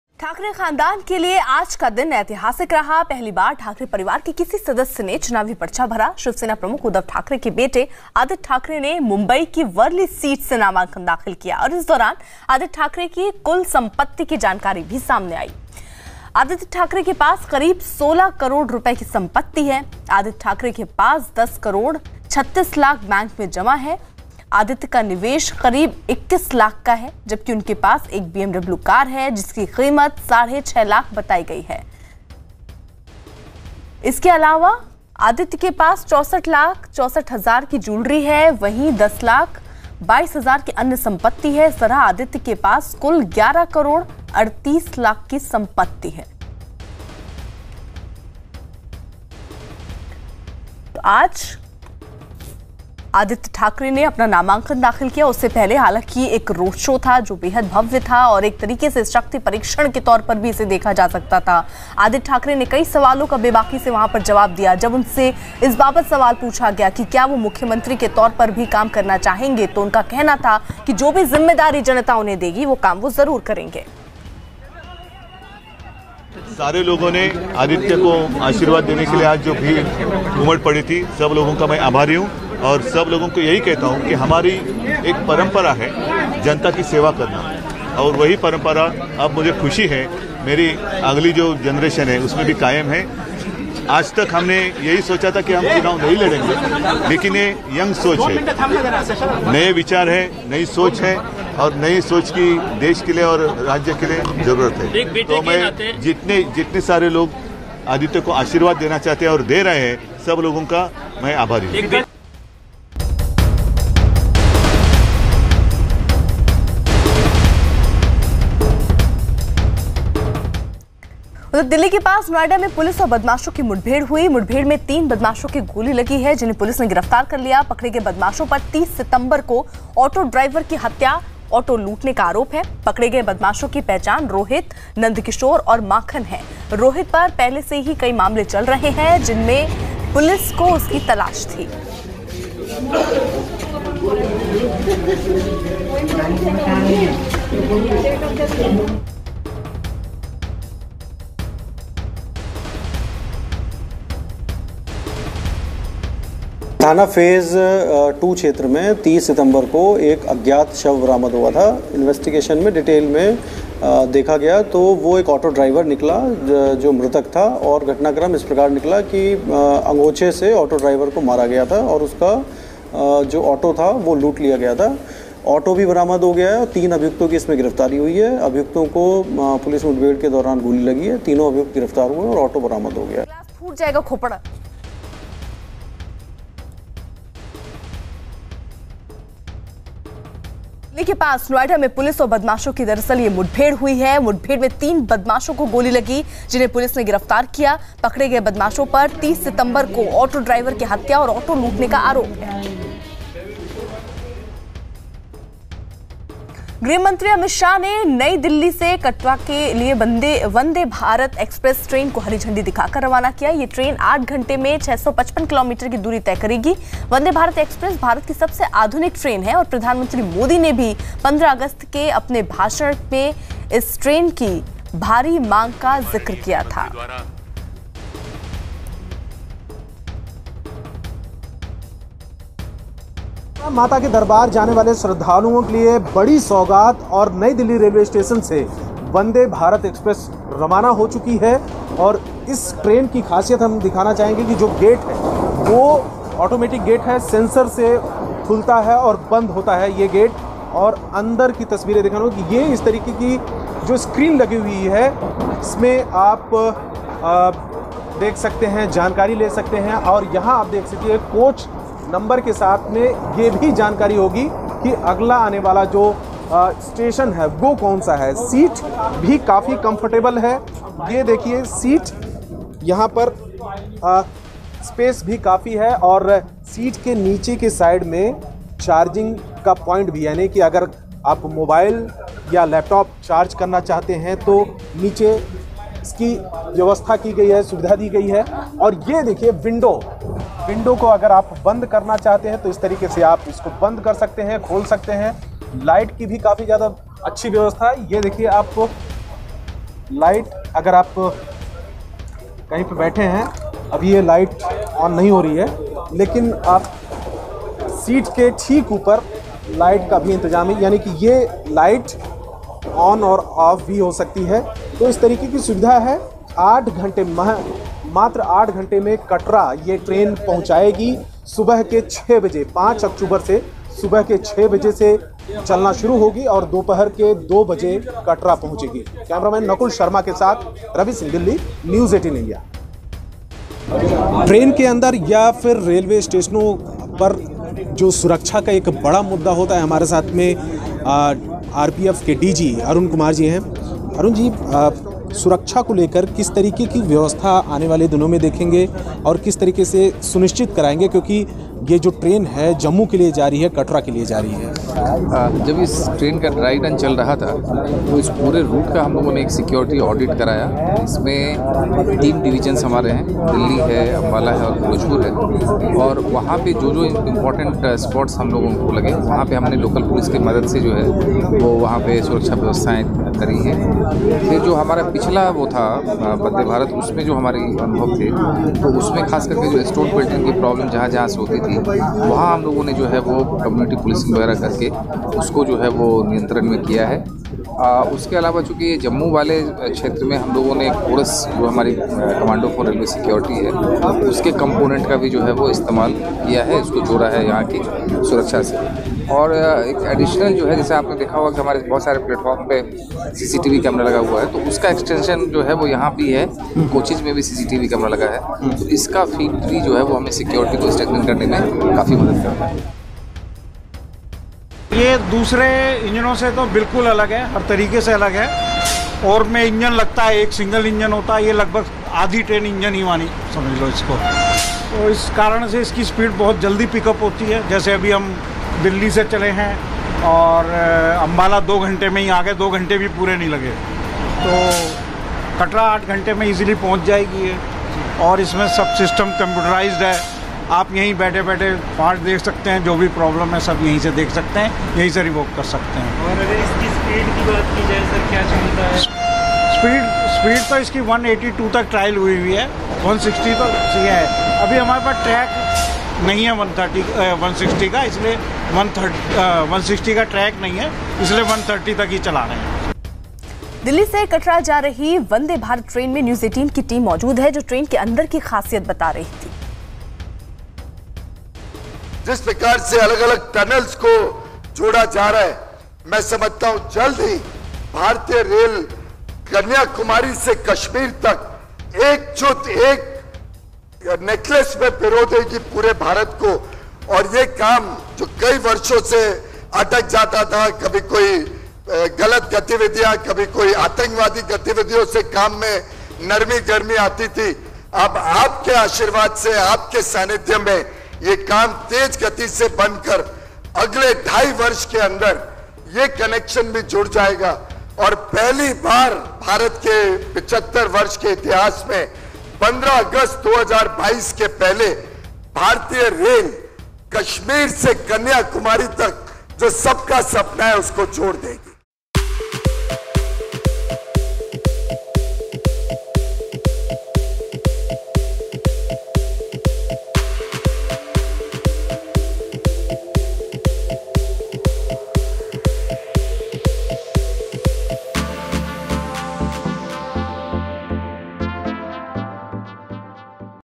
Aditya Thackeray ने भरा नामांकन, कुल संपत्ति की जानकारी भी सामने आई, करीब 16 करोड़ की संपत्ति
Maharashtra के आगामी चुनाव के लिए Aditya Thackeray ने आज अपना नामांकन भर दिया। उनके पास मौजूद संपत्ति की जानकारी सामने आई। उनके पास करीब 16 करोड़ की संपत्ति है।